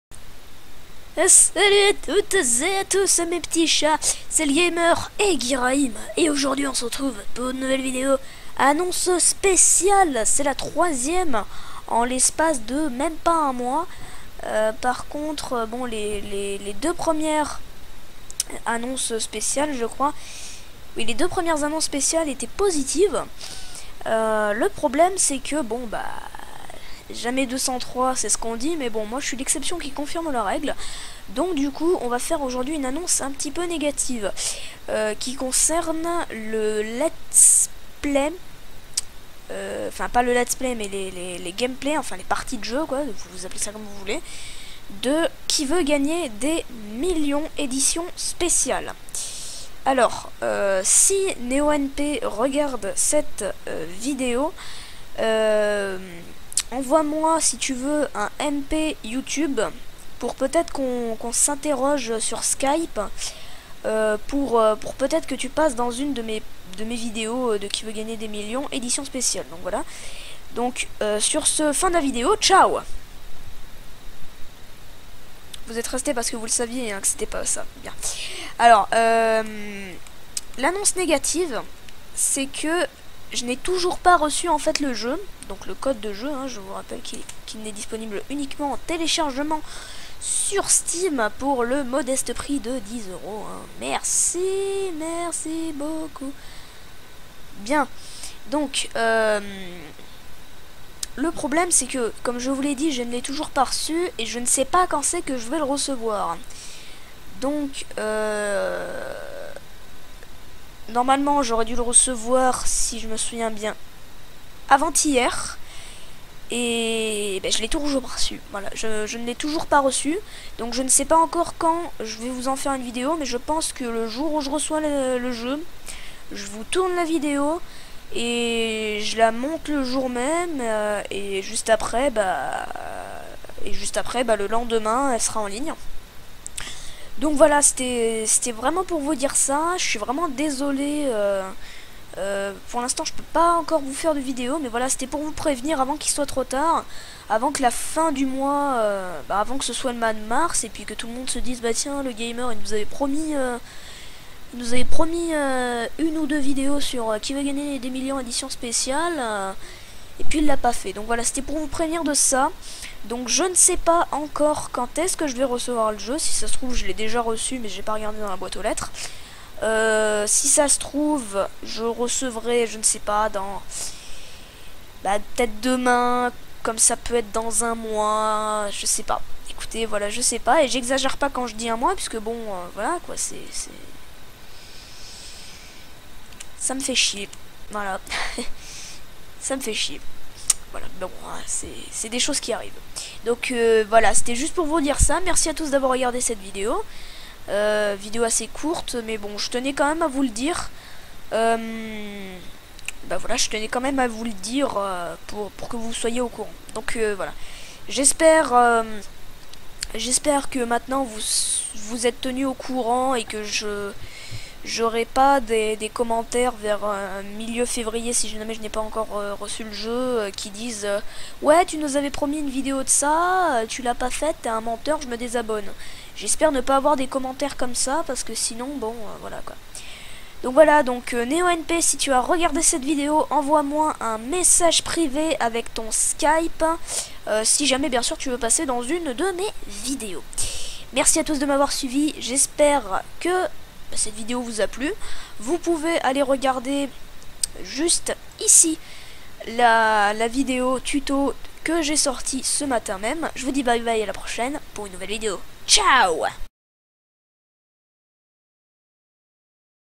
[Annonce spéciale] C'est pour quand QVGDM Éditions Spéciales ? Et bien...
3ème vidéo annonce, mais cette fois ci sur quelque chose de négatif (hélas).